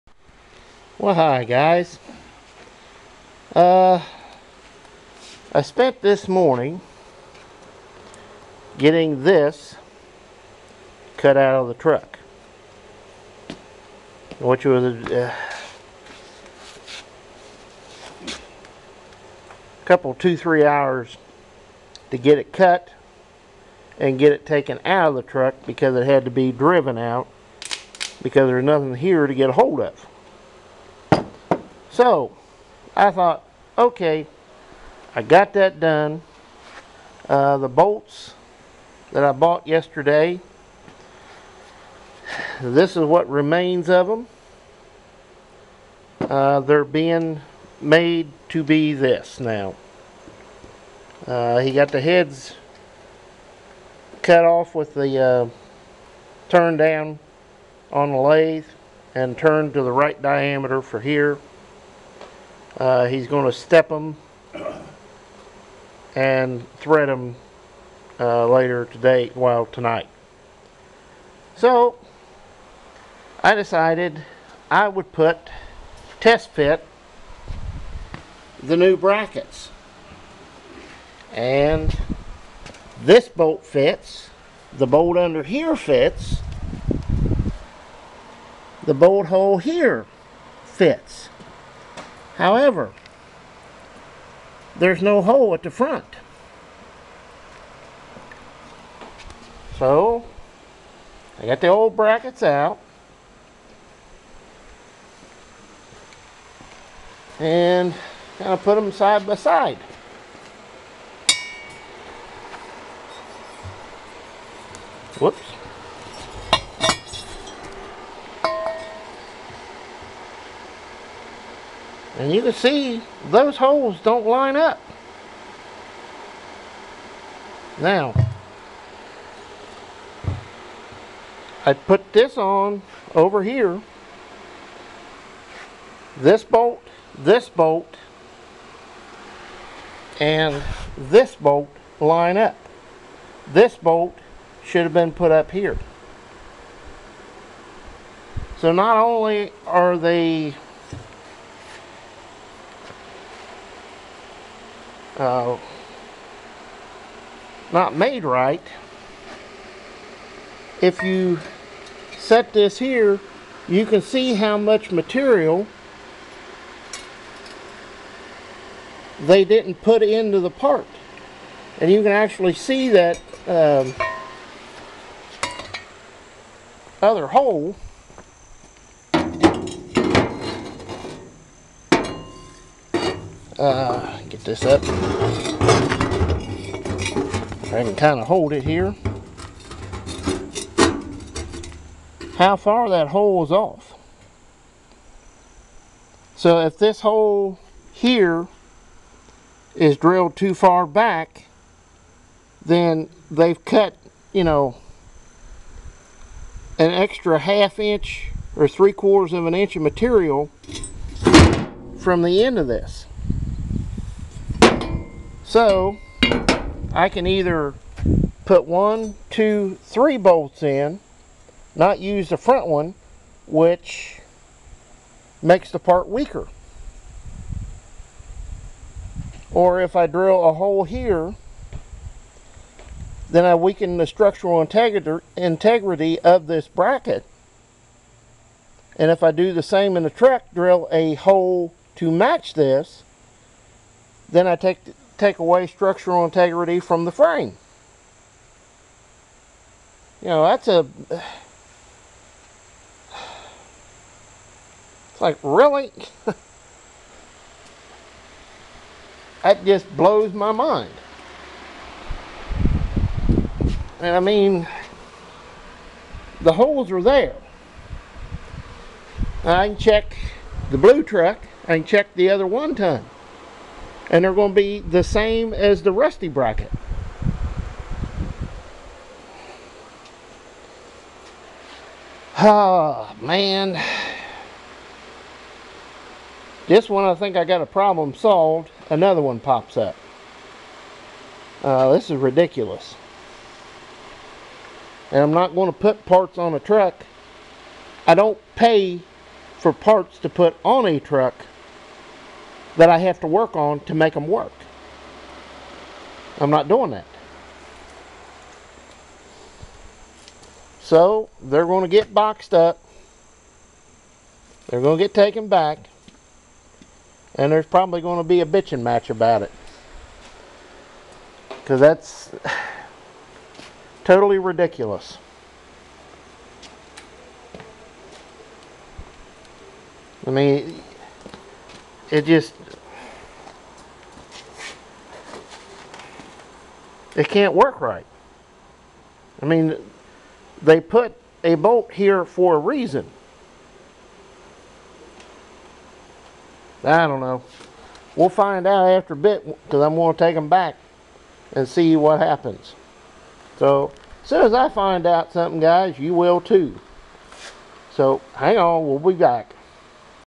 pt# 6 Working on Red and a New Problem !
After dropping off the bolts to my cousin last night to be made into the new pins I got to working on the engine some and did a test fit of the new radius arm brackets and found a whole new problem, They don't fit !  3 out of 4 bolts go in but the front bolt is drilled a 1/2 to short and there isn't enough material there to drill another hole in it so they got to go back and I know they might have a problem with it ! I hope not cause I'm not in the mood for it !  I cant believe this , But parts that they say are an exact fit !  Oh yell right ! I hope they take them back without a problem or there might be a fight !  Bandit